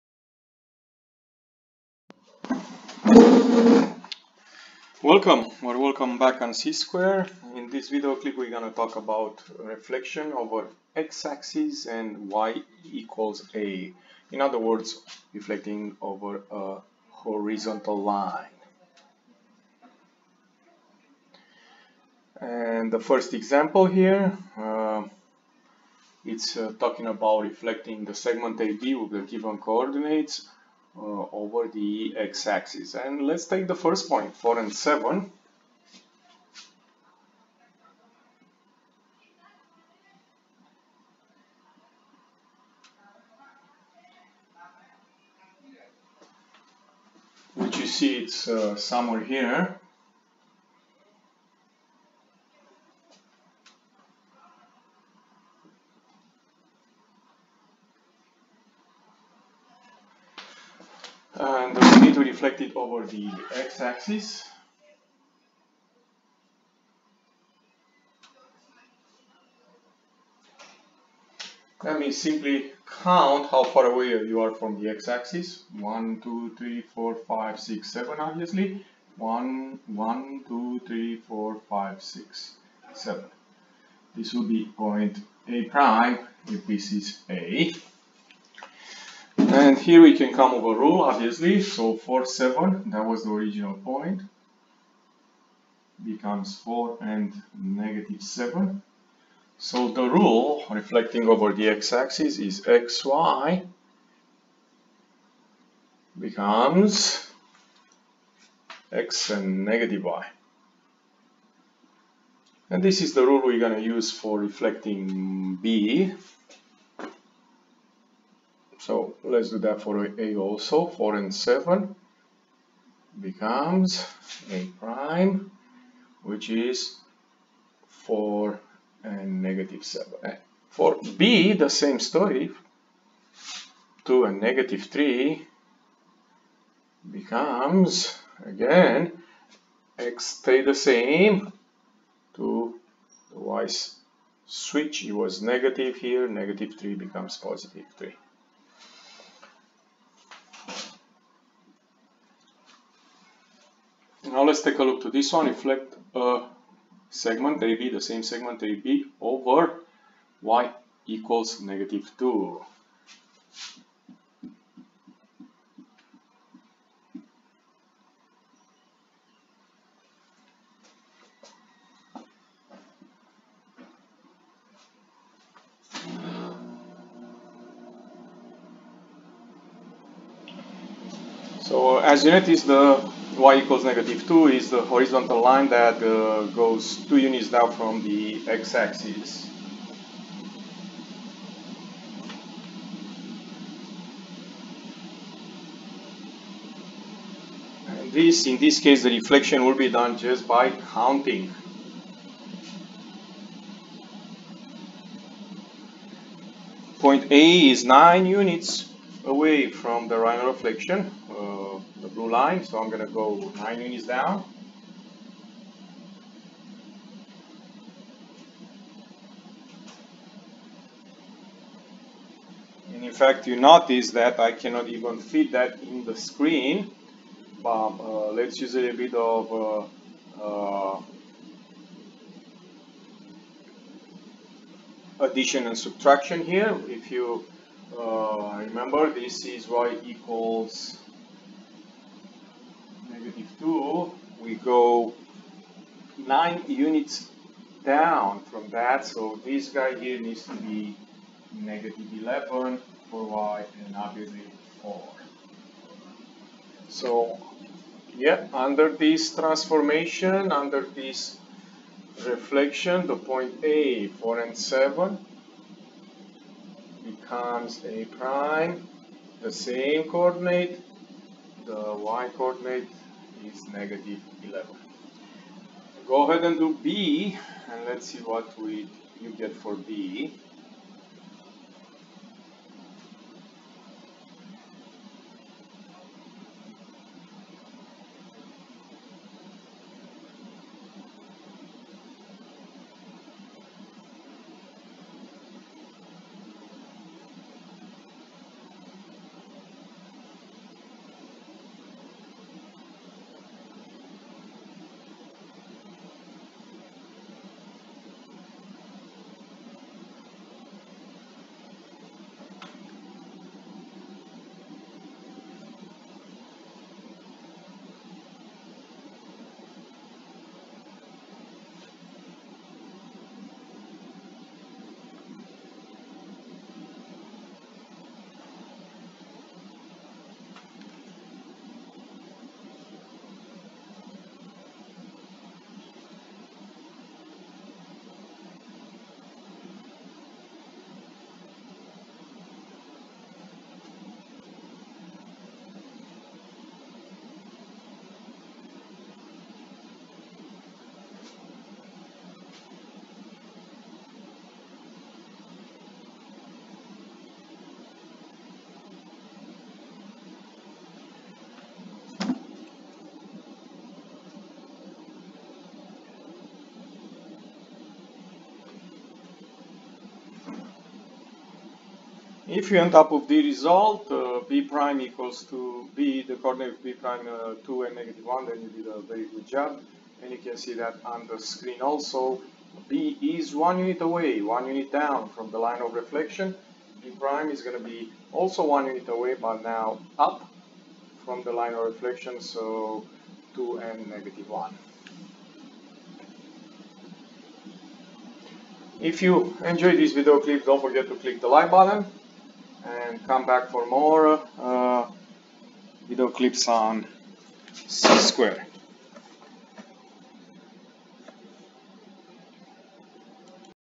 Reflection x axis and y=a
This clips covers reflection over x-axis or any horizontal line. It goes over graphical (counting) and analytical way (rule)